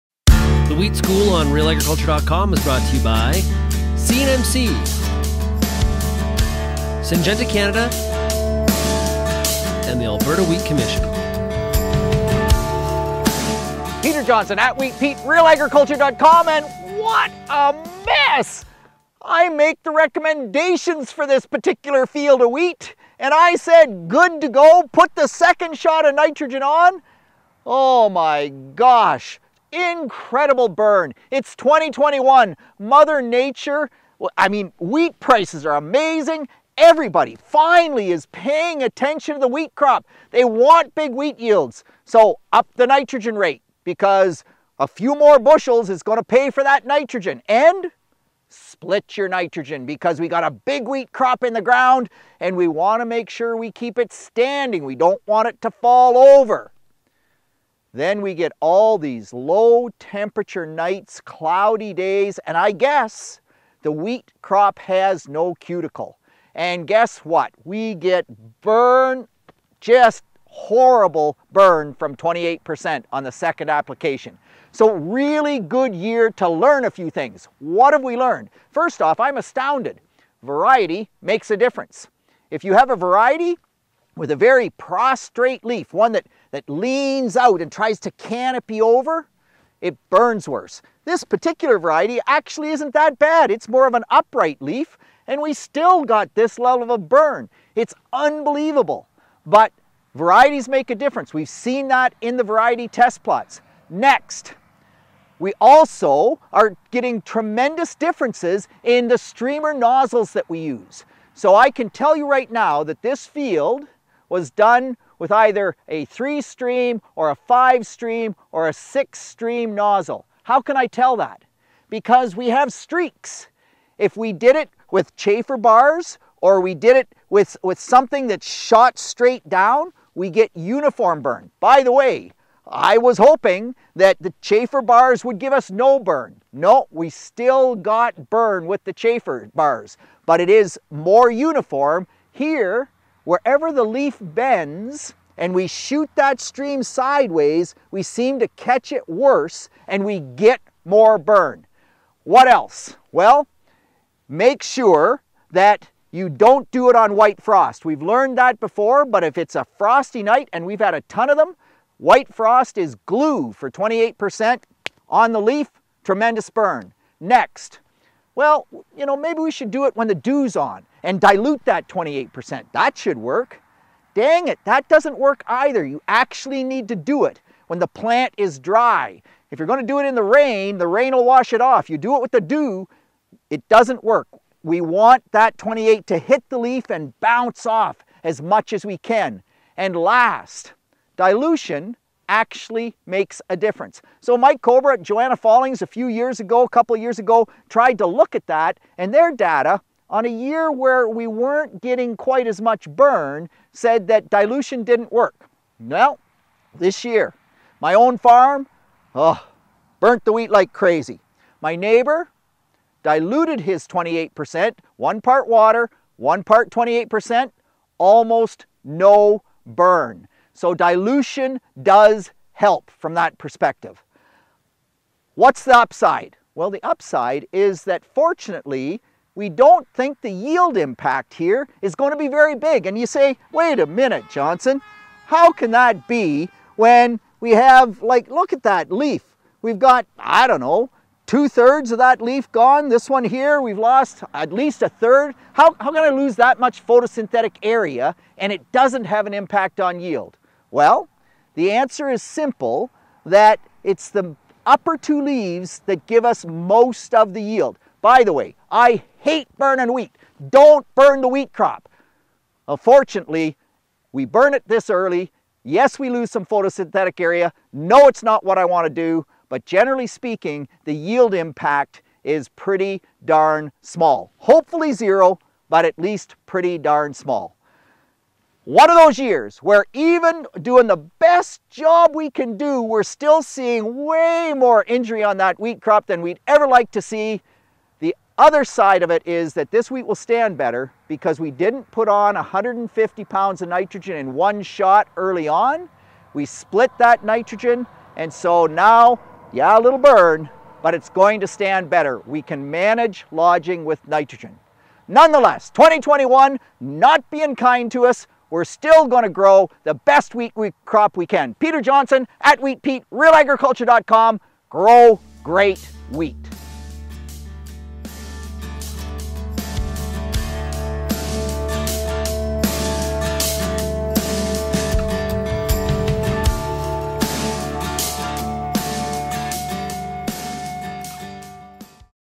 Wheat School: How to avoid leaf burn from UAN
Achieving big wheat yields means managing nitrogen, but split applications can damage wheat leaves.

Peter "Wheat Pete" Johnson is hopping mad in this Wheat School episode — mad at himself that is. In the video, he's standing in a wheat field with leaf-burn — a field that he said was good-to-go.

"Split your nitrogen, because we've got a big wheat crop in the ground and we want to make sure we keep it standing; we don't want it to fall over," says Johnson. However, low temperature nights, cloudy days, and the fact that the leaves don't have a cuticle, can cause horrible leaf burn from 28 per cent (UAN) on a second application, he explains.